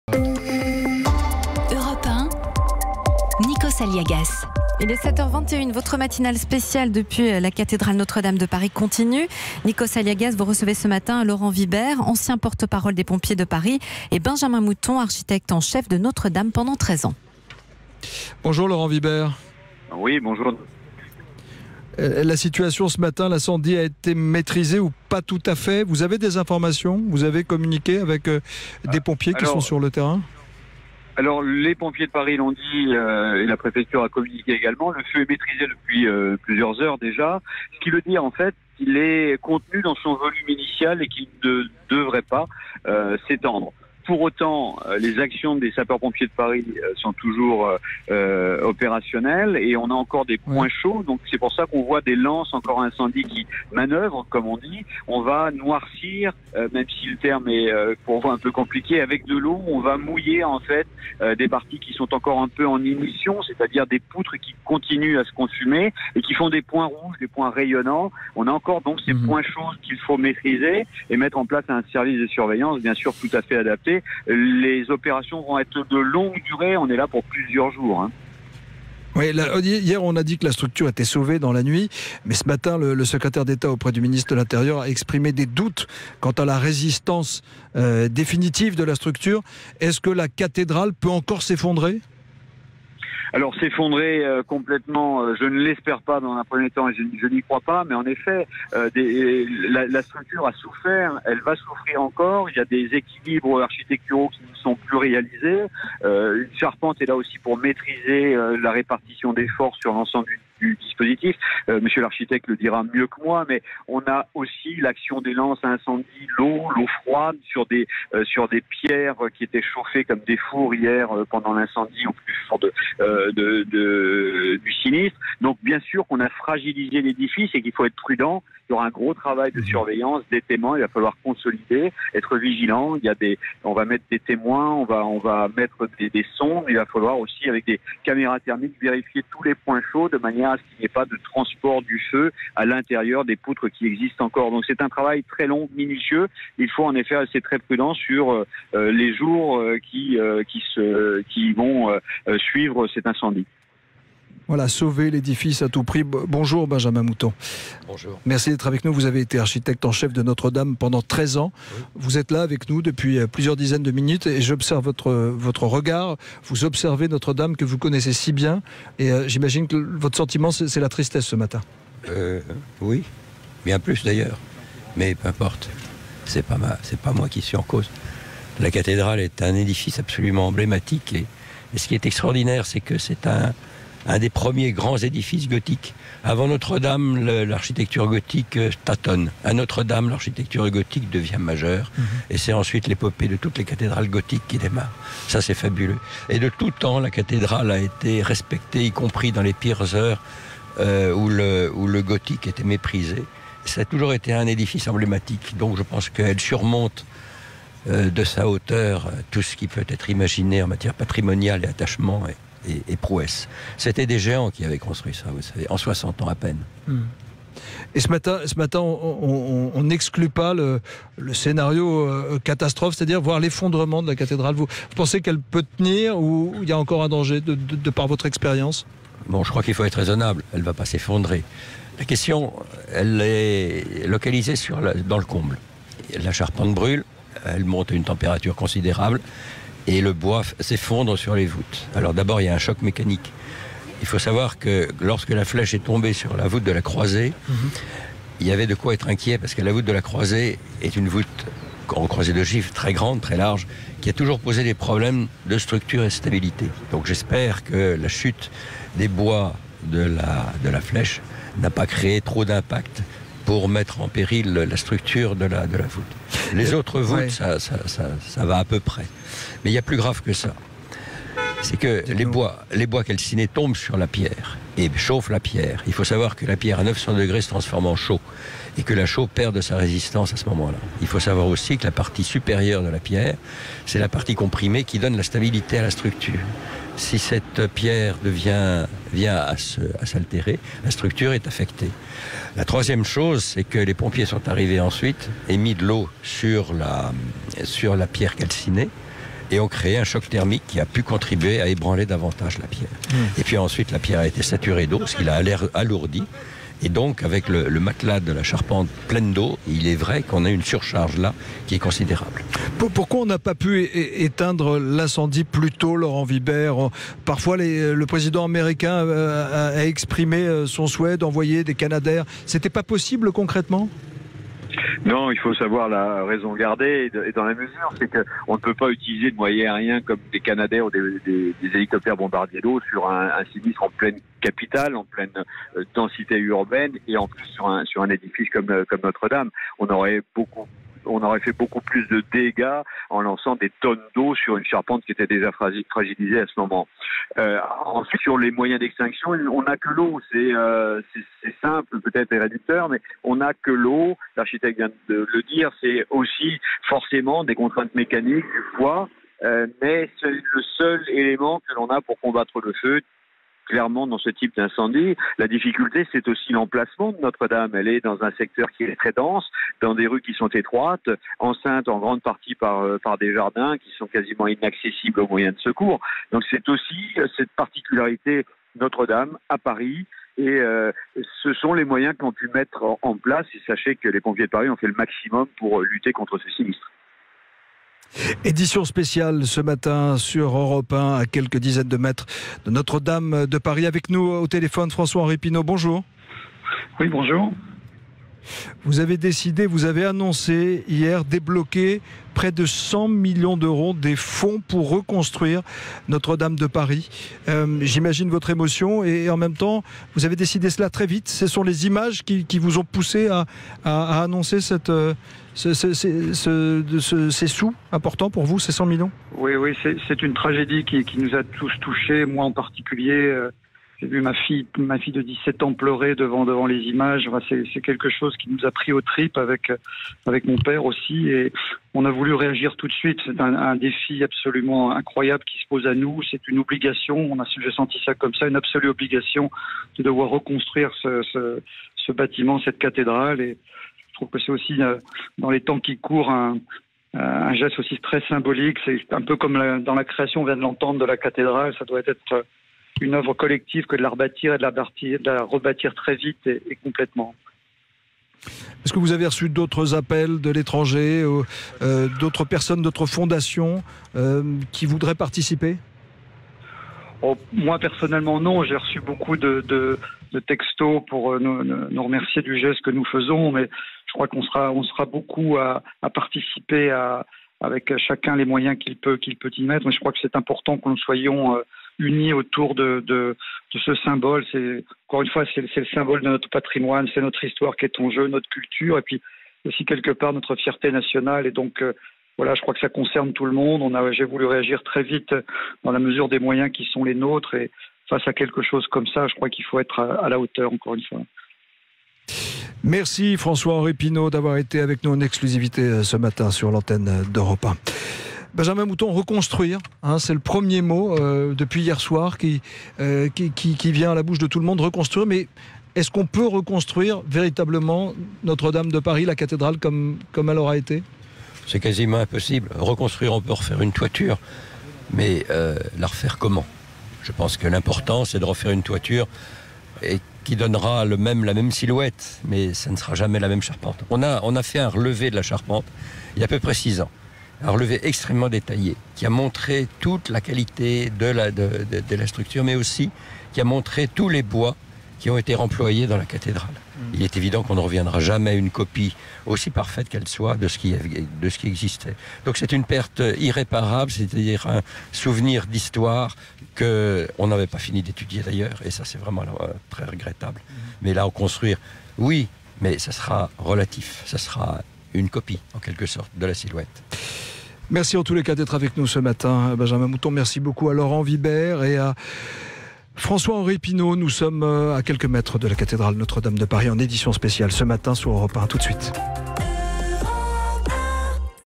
Reconstruction de Notre-Dame, la famille Pinault a voulu réagir très vite "dans la mesure des moy…
Au lendemain de l'incendie qui a ravagé Notre-Dame de Paris, Nikos Aliagas reçoit Laurent Vuibert, Benjamin Mouton et François-Henri Pinault pour parler de ce drame qui frappe cet emblématique édifice.